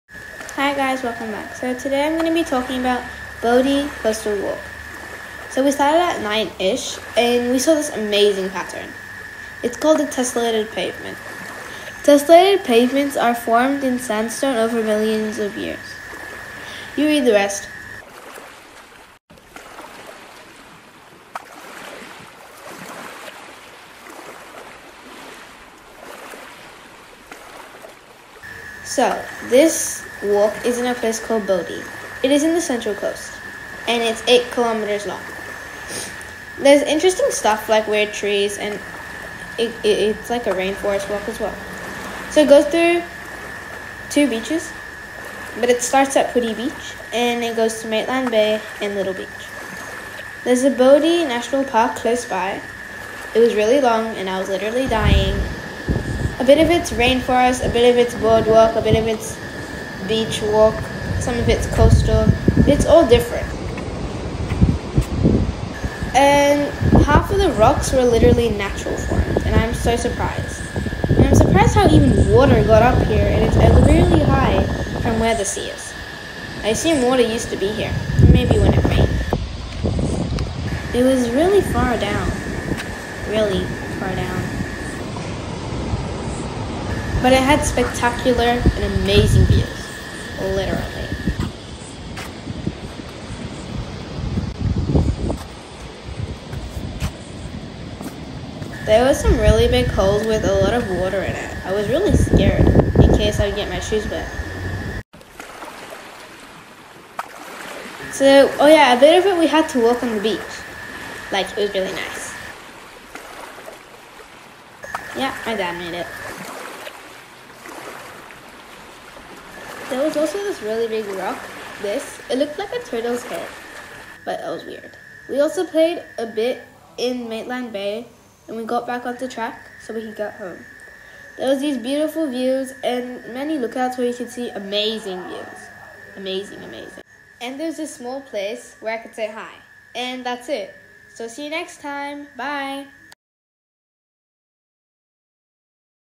Bouddi coastal walk
Bouddi Coastal Walk, NSW ~ Coastal walk in the Bouddi National Park which is about 8km.
Walk starting from the Putty beach goes to the Gerin Point lookout in 1 km from there taking us to Maitland Bay in 1.6 km.
Walking for another 3-4 km from Maitland Bay  to finish at Little beach.